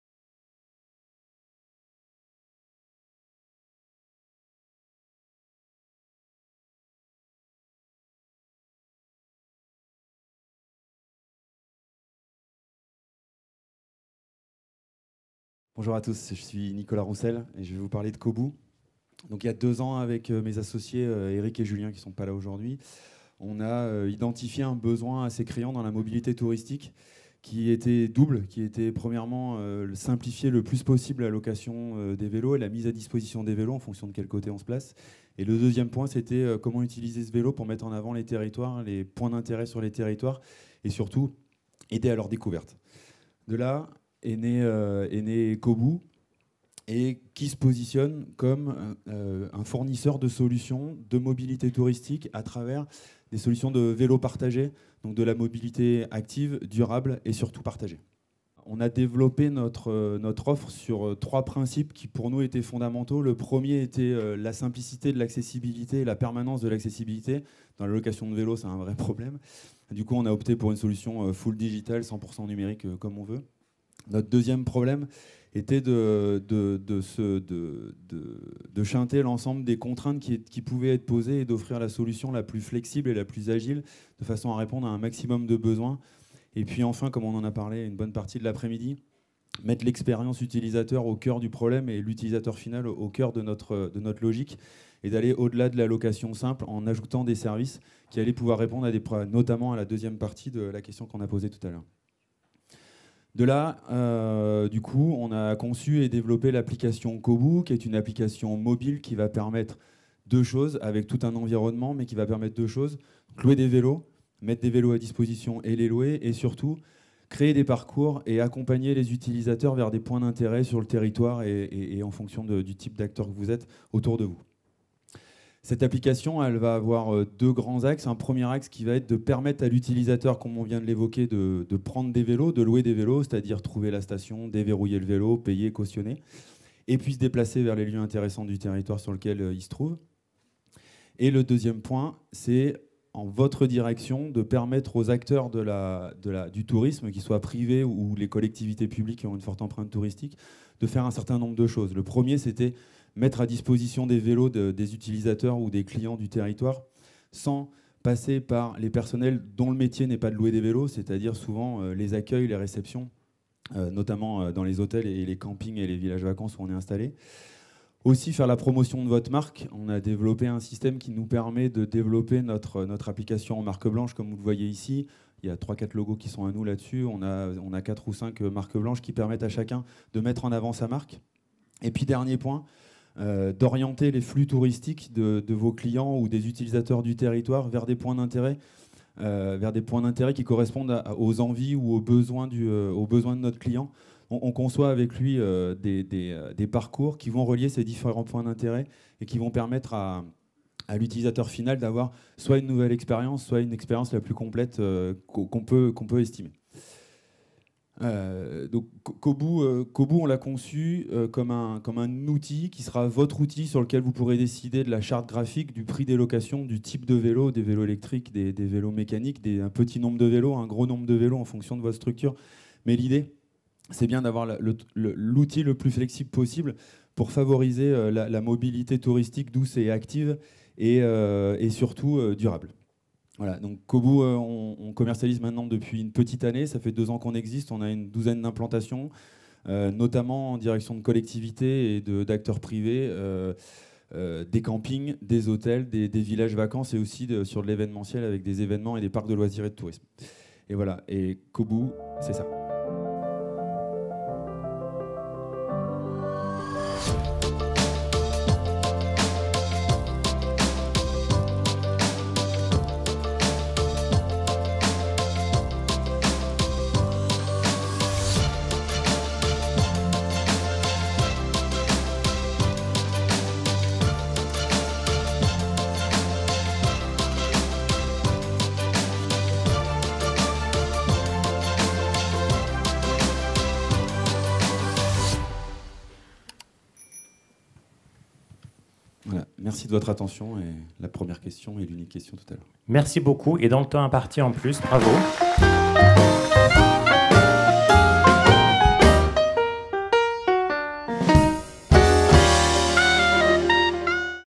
StartUp Contest Next Tourisme 2019 : le pitch de Koboo
Dans le cadre du Start-Up Contest qui a suivi la 6e conférence Next Tourisme le 23 mai 2019